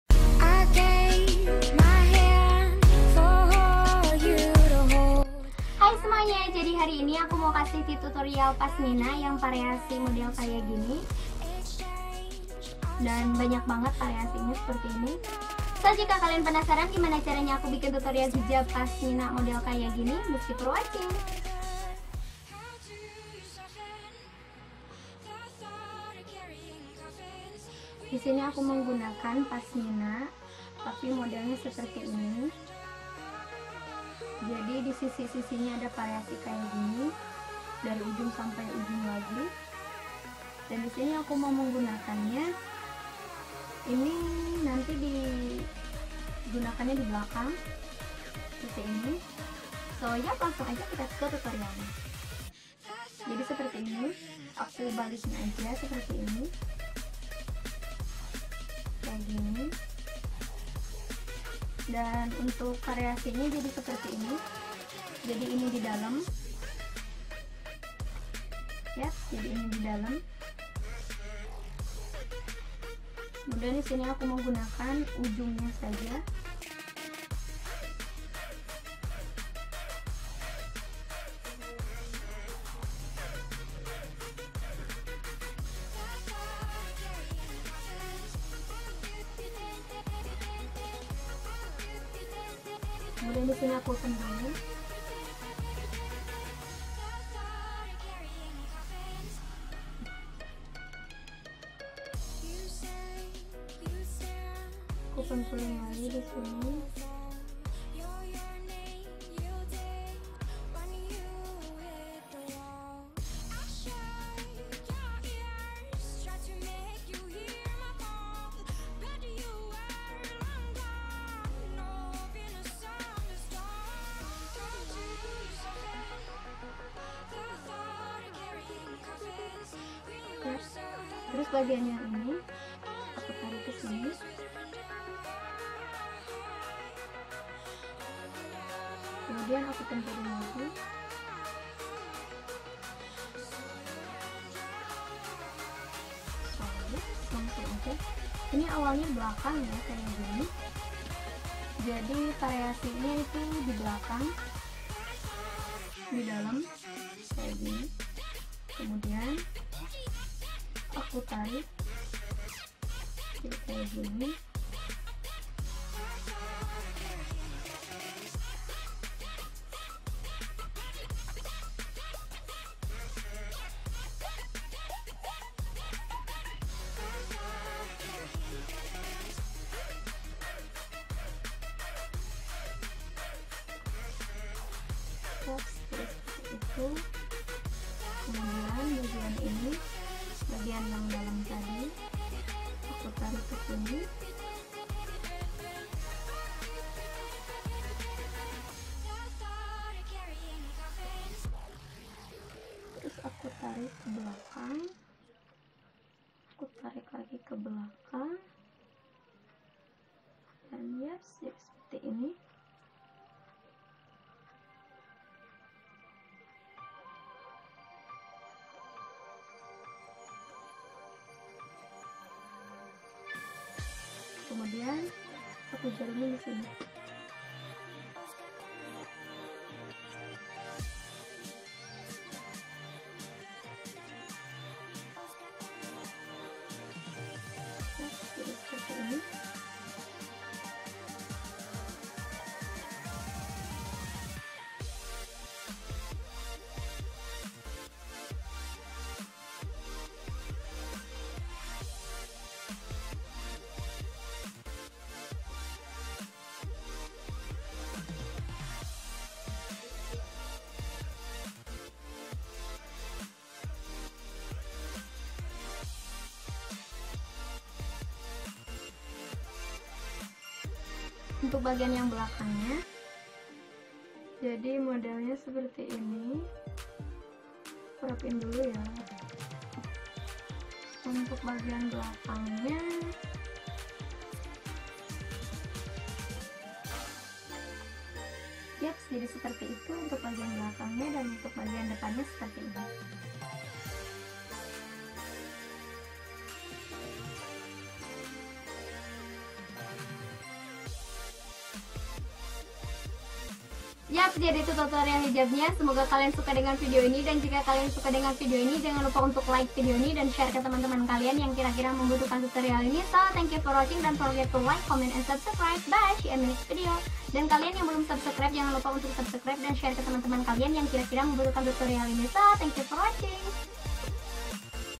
TUTORIAL HIJAB [ PASMINA] Dengan Beragam Pariasi Dan Model Kekinian 2 1
cara pake hijab pasmina simple,elegan, dan mewah.
cocok banget buat acara ke pesta.kondangan dan ok buat di pake sehari2
Dan bagi kalian suka dengan model hijab dan bajunya silahkan klik link dibawah ini